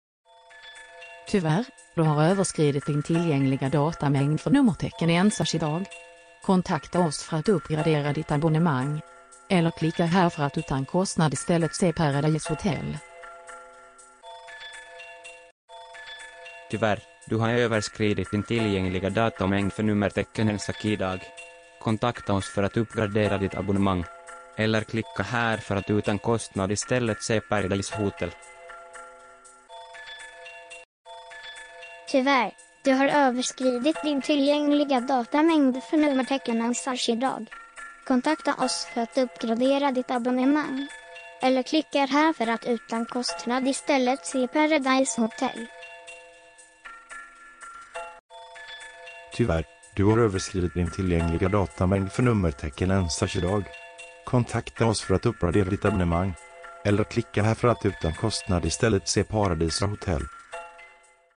109. Amerikanskt beslut riskerar döda vårt öppna och fria internet
… eller klicka här för att utan kostnad istället se Paradise hotel.]

Googla nätneutralitet. Informera dig, diskutera, var beredd att agera. Se ensakidag #108 för bakgrundsinformation.